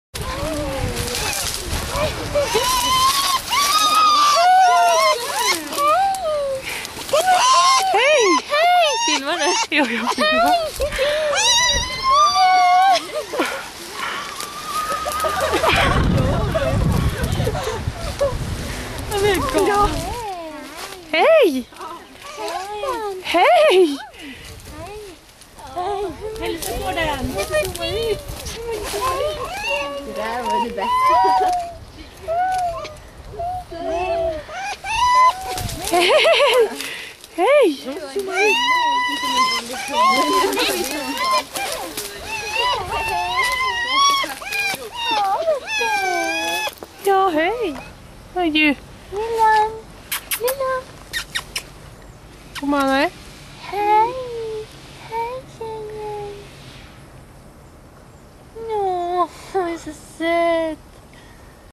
Världens sötaste räv.
På Lycksele djurpark, helt underbart söt.